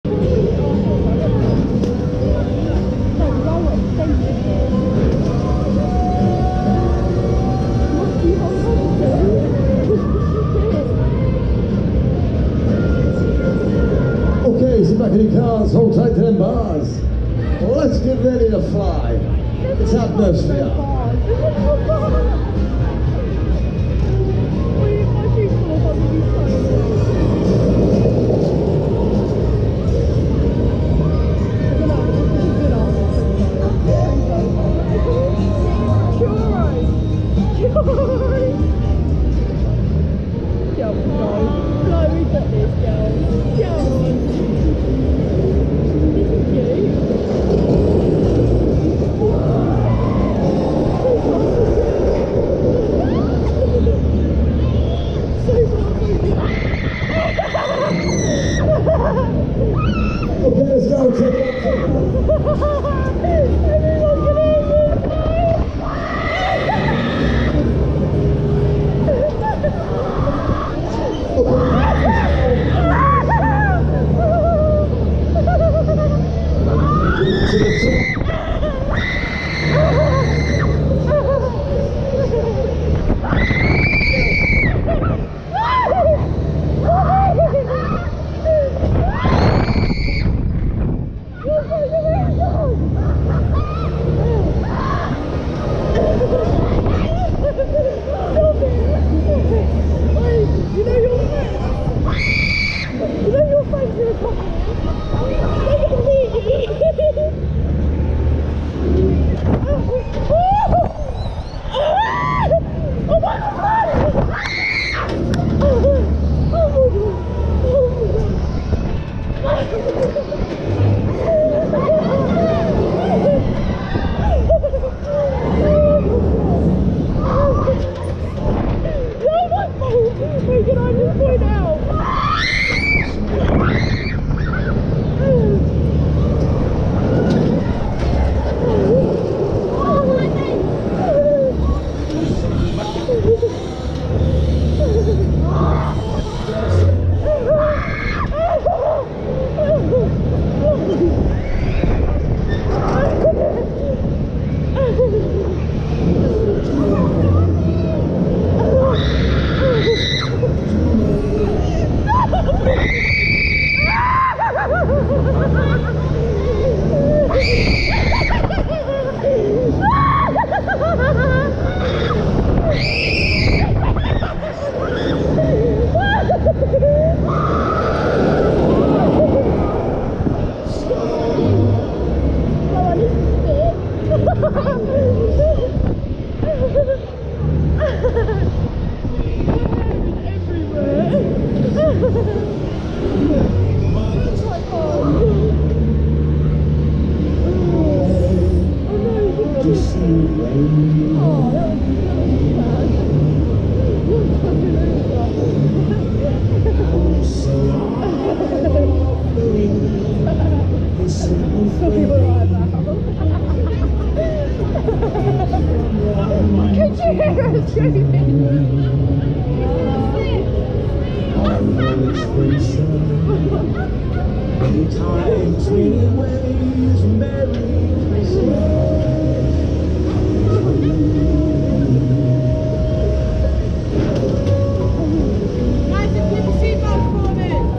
AtmosFear (S&D Leisure) On Ride Hyde Park Winter Wonderland 2021
Technical Park Loop Fighter